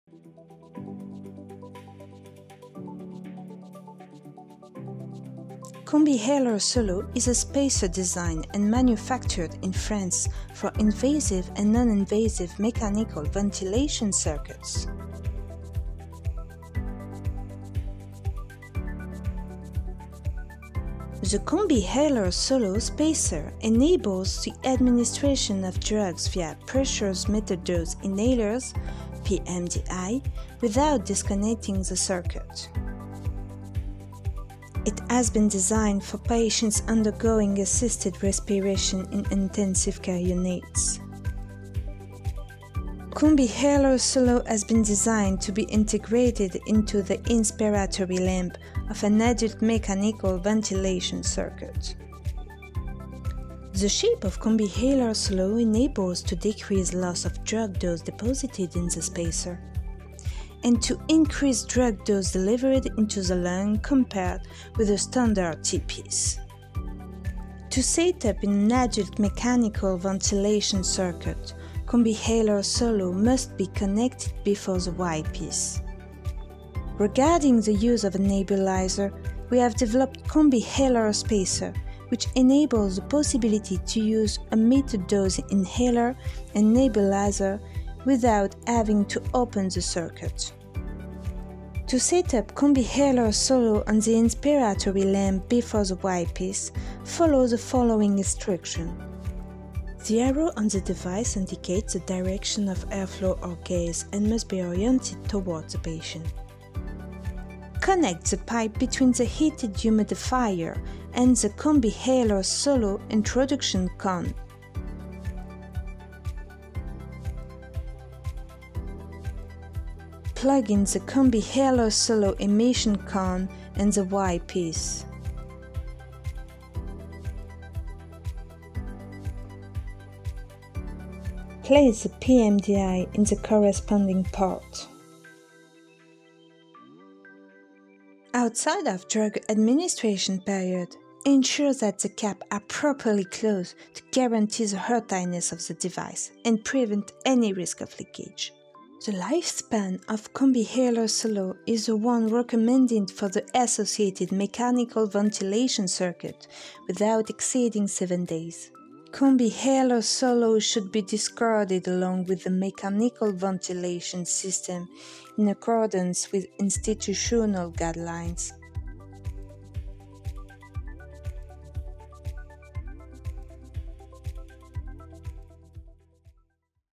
How to connect CombiHaler Solo to an adult mechanical ventilation circuit ?
CombiHaler® Solo is a spacer designed for invasive and non-invasive mechanical ventilation circuits. The CombiHaler® Solo spacer enables the administration of drugs via pressurized-metered-dose (pMDI) inhalers without disconnecting the circuit.
CombiHaler Solo is designed and manufactured in France by the OptimHal laboratory, under the ISO 13485: 2016 process (manufacturing standard for medical devices). It has been designed for patients undergoing assisted respiration in intensive care units.

CombiHaler® Solo is a Class IIa, CE 0459 medical inhalation device.